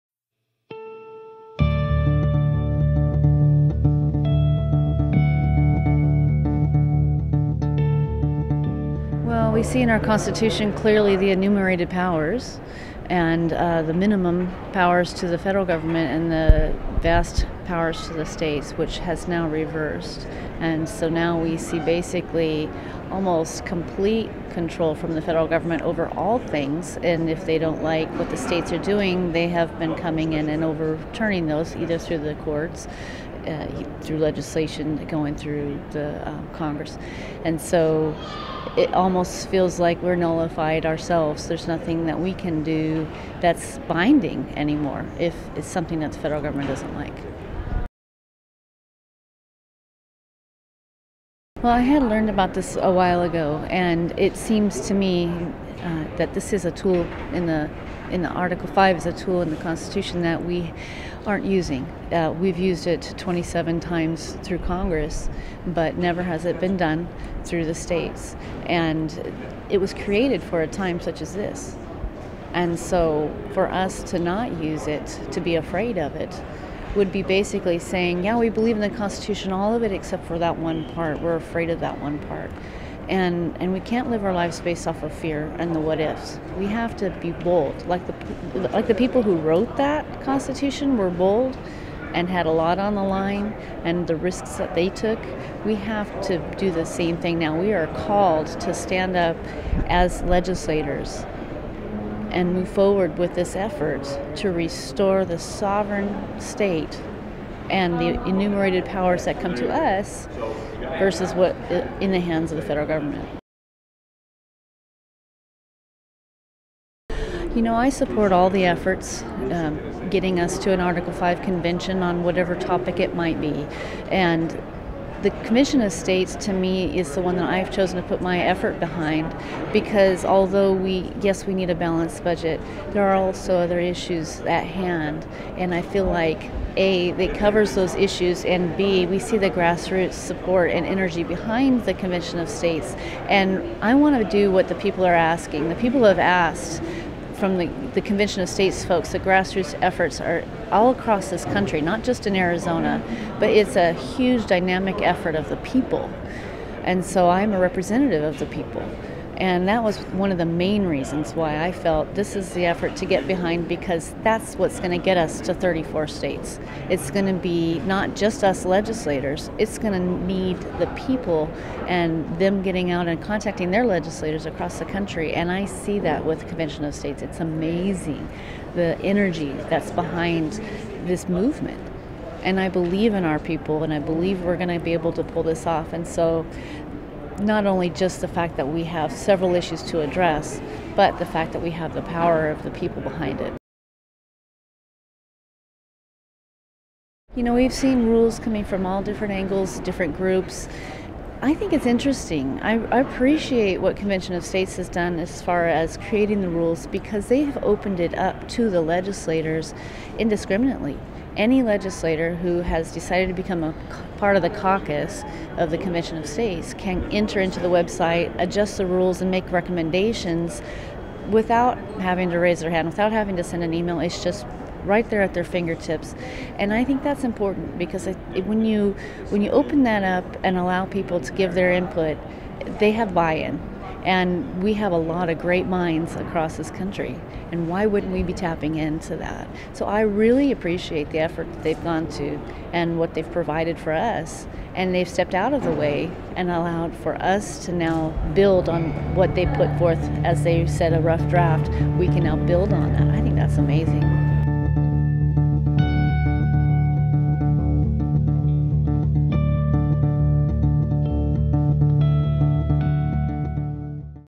Interview with Arizona Rep. Kelly Townsend
Arizona State Representative Kelly Townsend answers questions like, Why do you support Convention of States and How will the Convention of States Caucus help the movement?